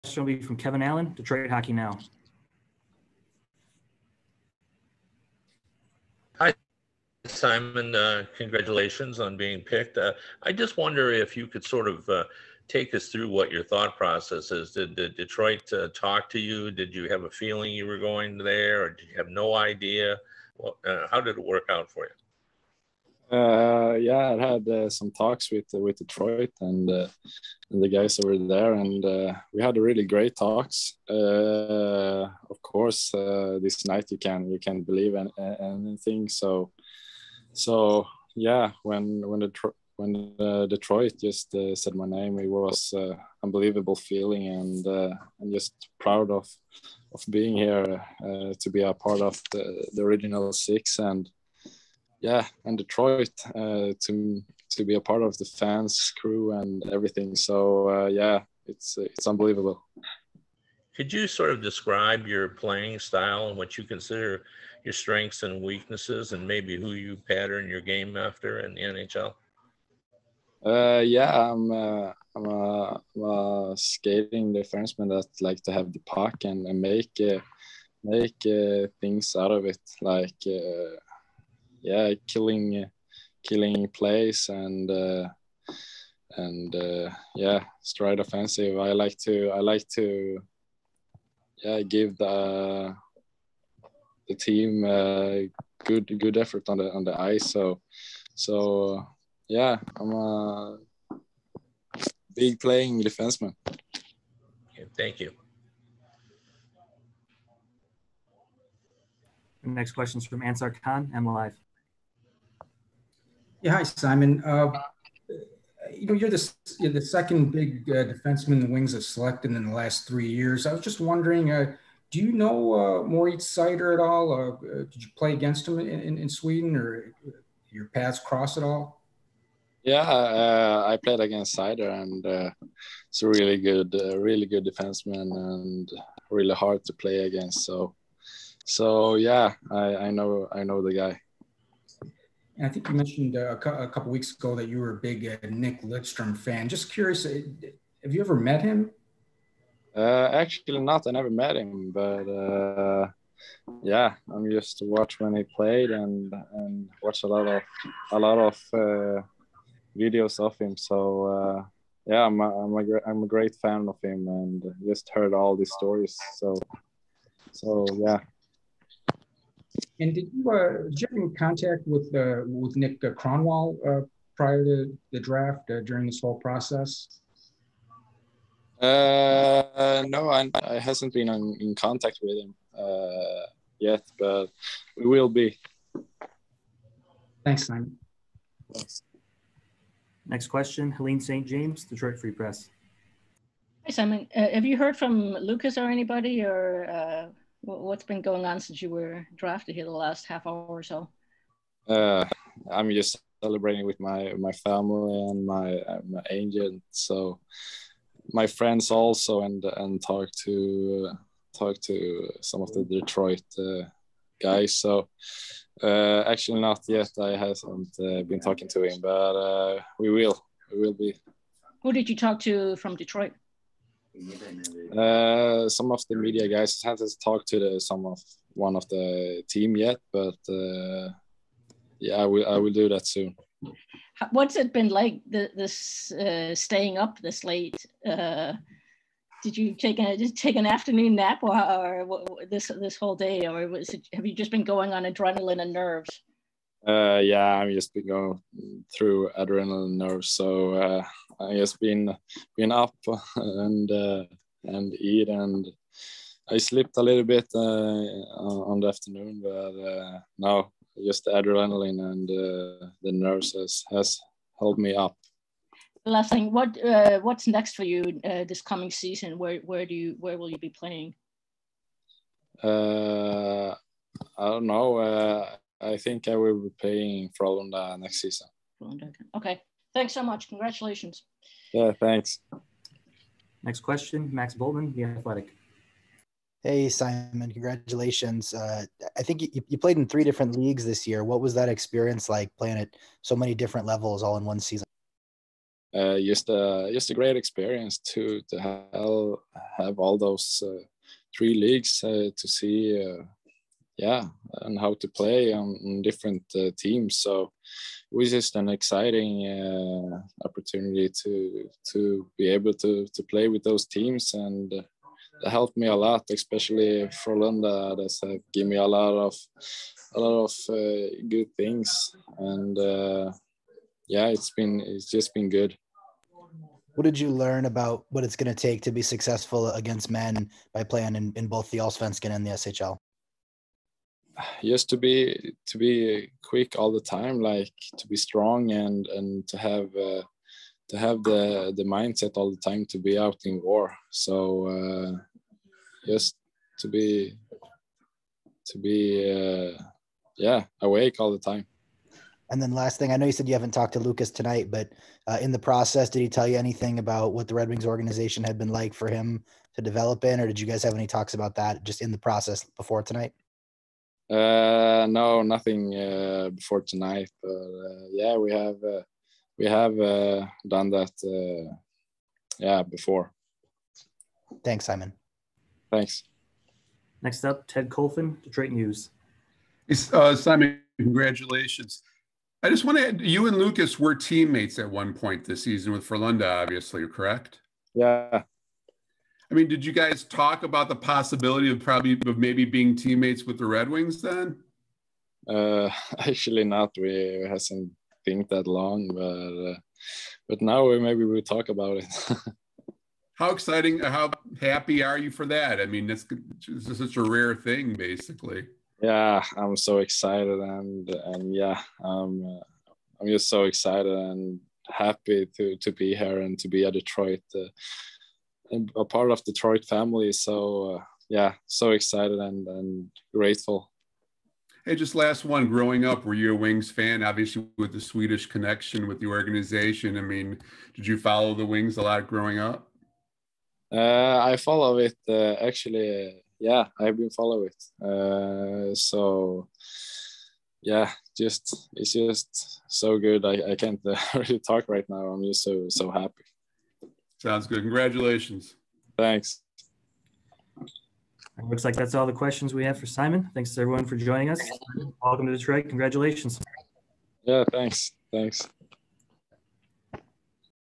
Simon Edvinsson | 2021 NHL Draft Media
Simon Edvinsson speaks with the media after being selected sixth overall by the Detroit Red Wings in the 2021 NHL Draft.

Presented by Meijer